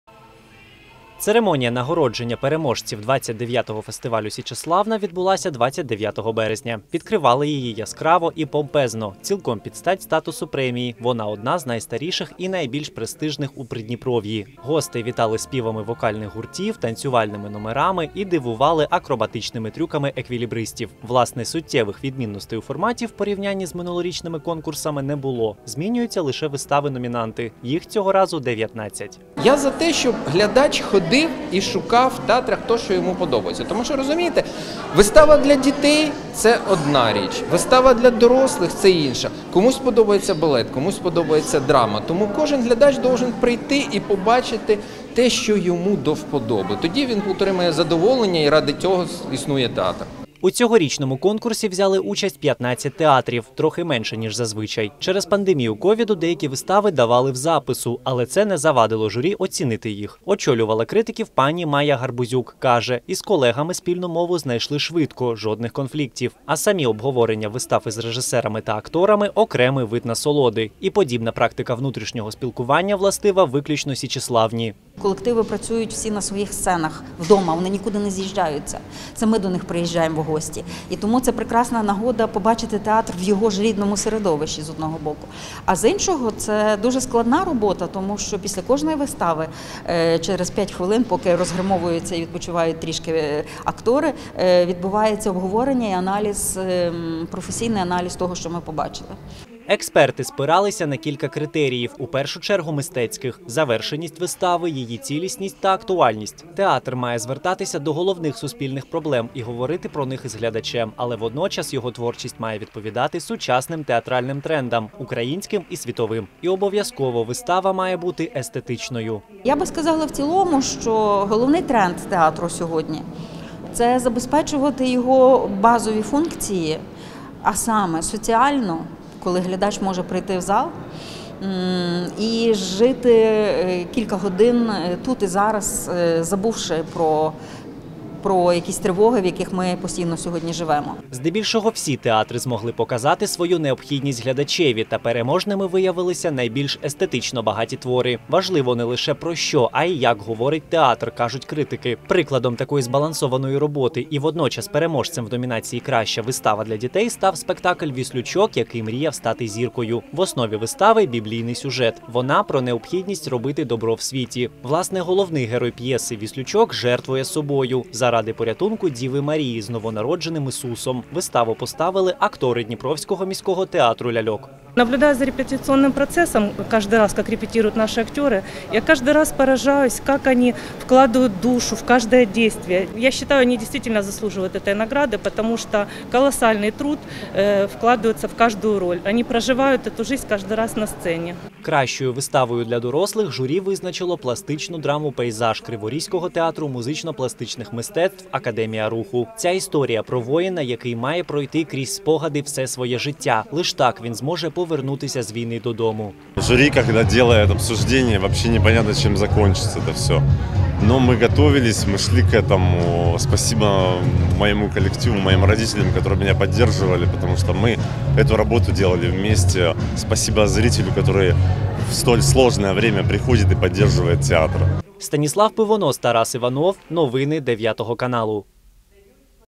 Театральний фестиваль «Січеславна-2021» добіг кінця: хто отримав гран-прі?
Театральний фестиваль «Січеславна-2021» добіг кінця.
 В ньому взяли участь 15 професійних і молодіжних театрів Запорізької та Дніпропетровської областей. Усі вистави фестивальної програми – виключно прем’єри сезону.
Найкращими виставами відзначилися одразу два театри. Хто ж вони – володарі гран-прі найповажнішого конкурсу Придніпров’я – дивіться у сюжеті.